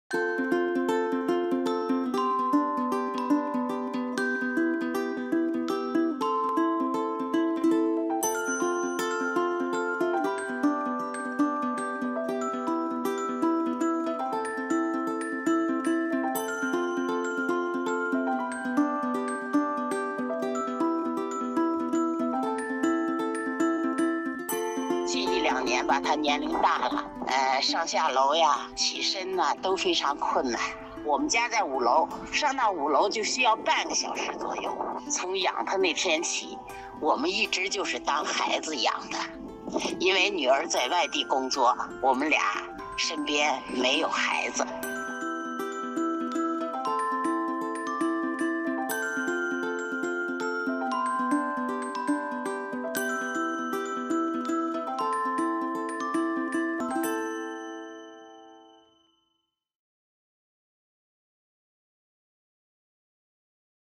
Elderly Couple Carry Aging Dog Downstairs
A 65-year-old woman and her husband’s commitment to caring for their aging chow chow, who has trouble standing, includes carrying the dog up and down five flights of stairs twice a day.